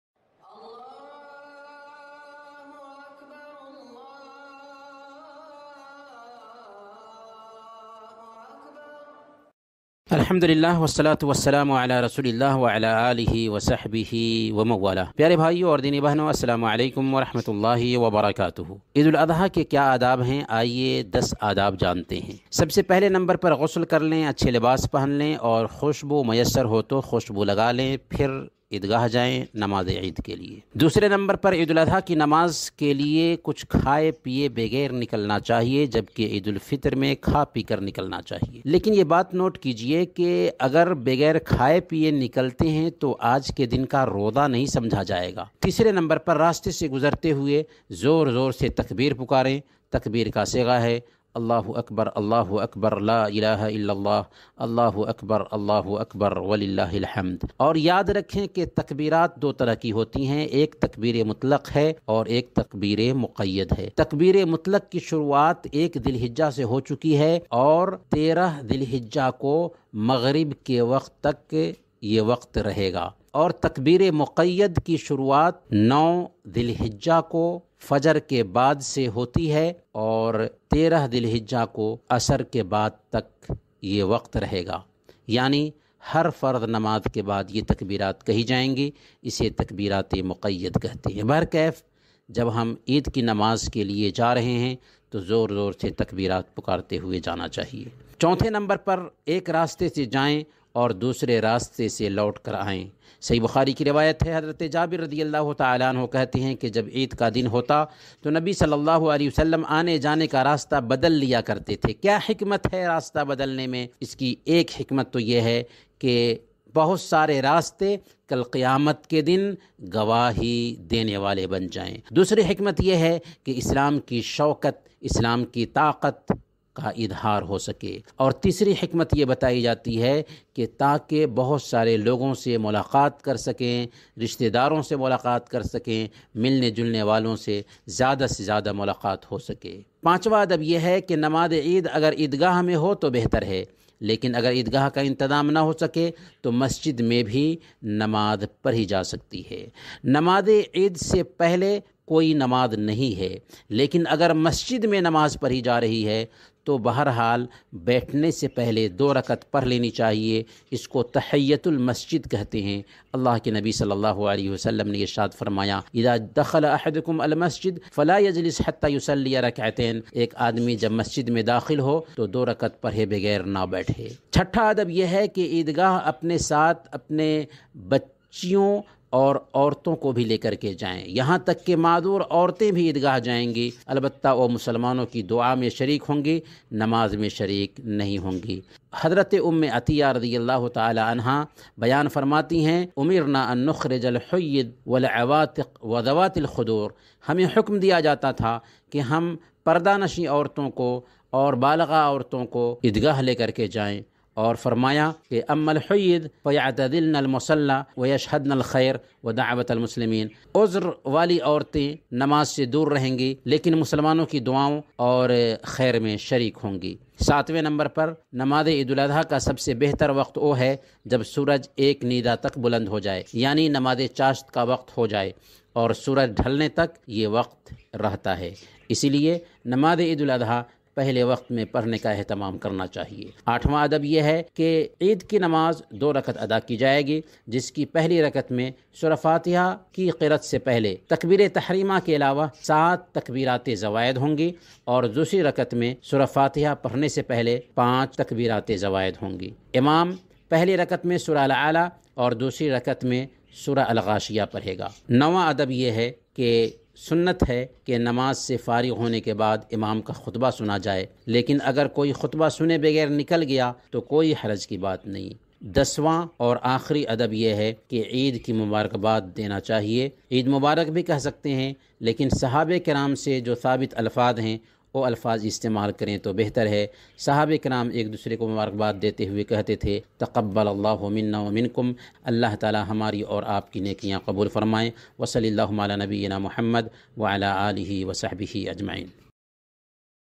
Eid-ul-Azha k Aadaab-o-Masail | Sheikh Safat Alam Taimi | 24SevenIslam
Eid-ul-Azha k Aadaab-o-Masail | Sheikh Safat Alam Taimi | 24SevenIslam | Authentic Islamic Lectures

عید الاضحیٰ کے آداب و مسائل
شیخ صفات عالم تیمی